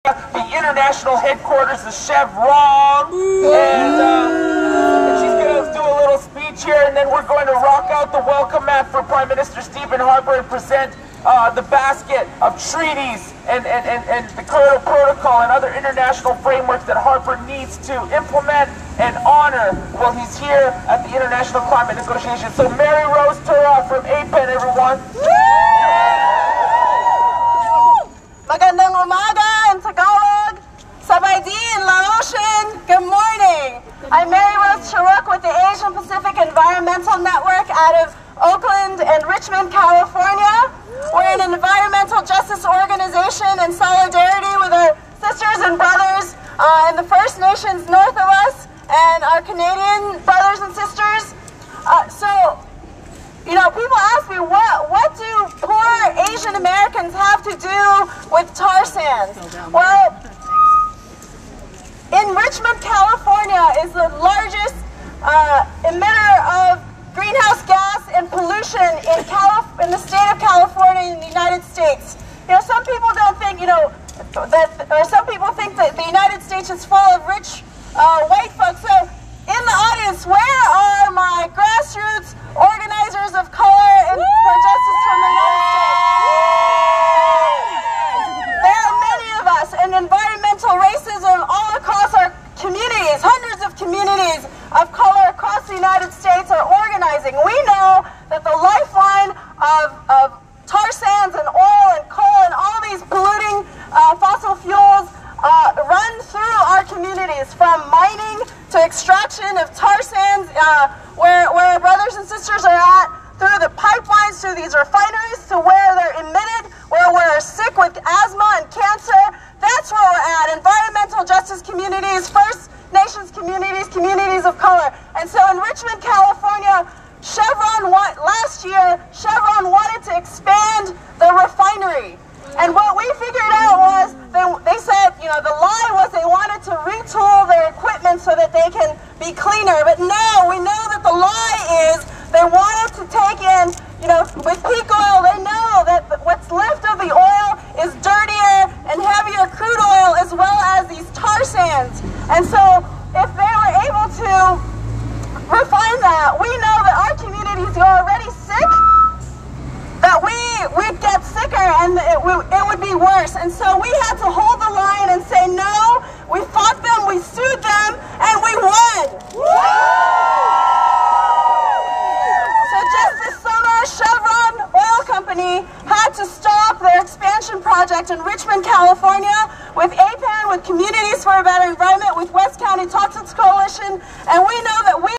Mari Rose Taruc, APEN in Copenhagen at Chevron International Headquarters
Mari Rose Taruc is from Asian Pacific Environmental Network speaks at the international headquarters in Copenhagen. Mari Rose,  spoke on a campaign in Richmond, CA where Chevron is emitting the largest amount of green house gas and pollution in the state of California.  APEN, Communities for a Better Environment and West County Toxic Coalition stopped the expansion of the Chevron refinery in Richmond CA.